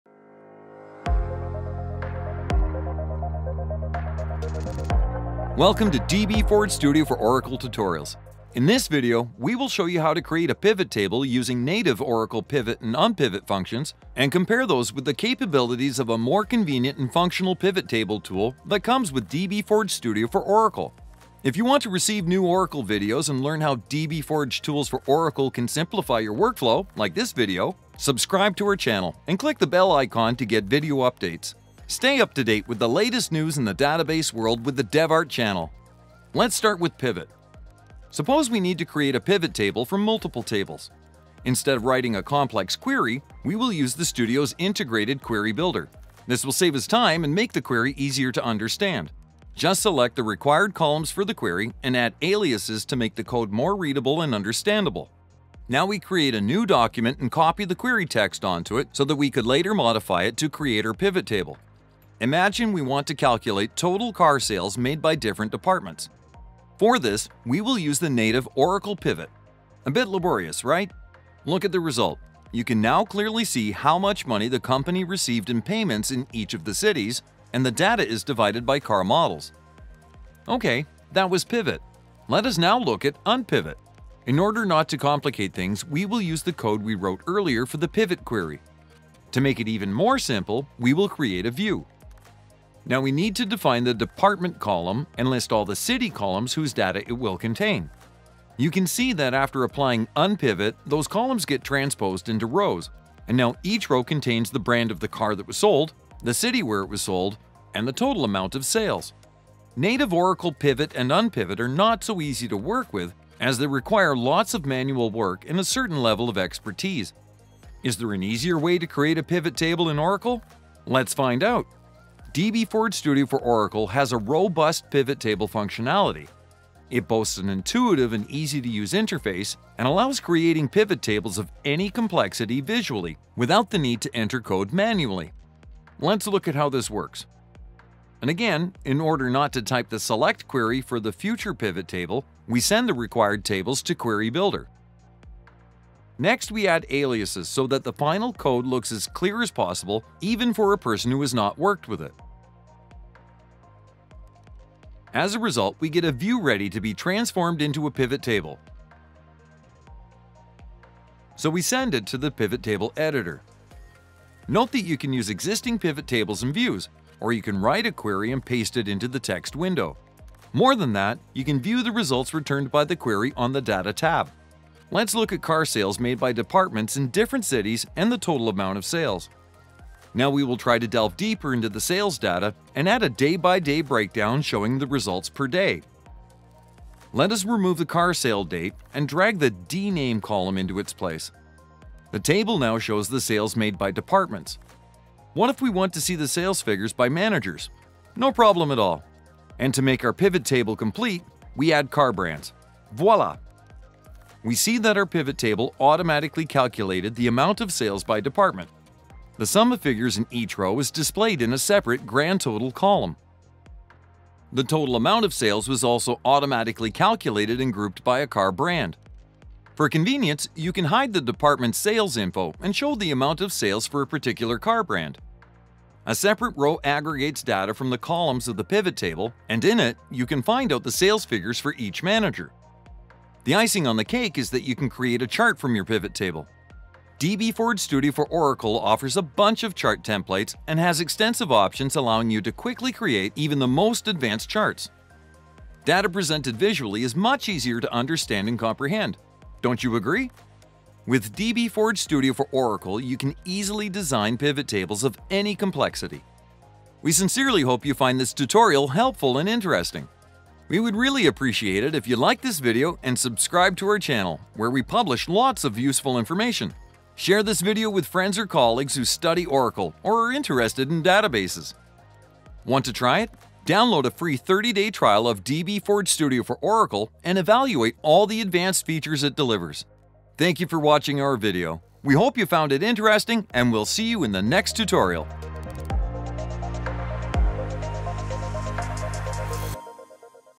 Create PIVOT Tables and UNPIVOT Data in Oracle Effortlessly using dbForge Studio
In this video, we will show you how to create a pivot table using the native Oracle PIVOT and UNPIVOT operators and compare them to the capabilities of a more convenient and functional pivot table tool that comes with dbForge Studio for Oracle.

Timestamps:
0:00 Introduction
0:36 PIVOT
1:22 UNPIVOT
1:58 Easier way to create Pivot Table in Oracle
2:41 Using Pivot Table Editor
3:57 Creating a chart from Pivot Table